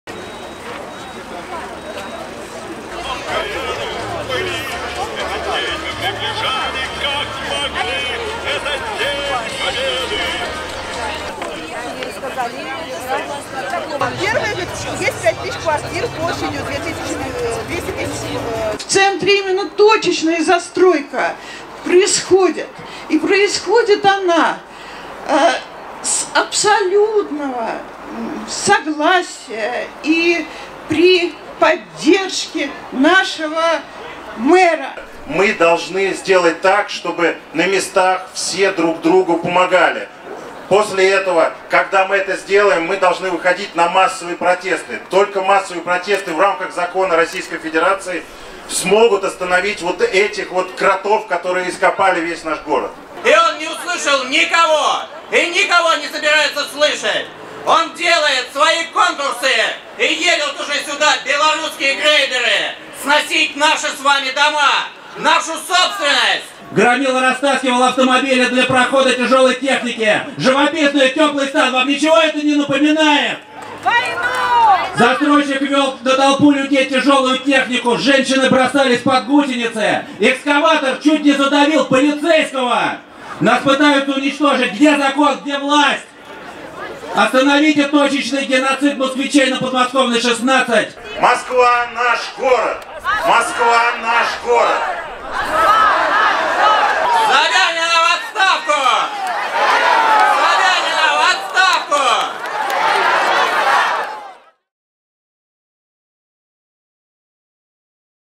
Митинг на Суворовской площади
Митинг "За права москвичей" прошёл на Суворовской площади 27 мая 2017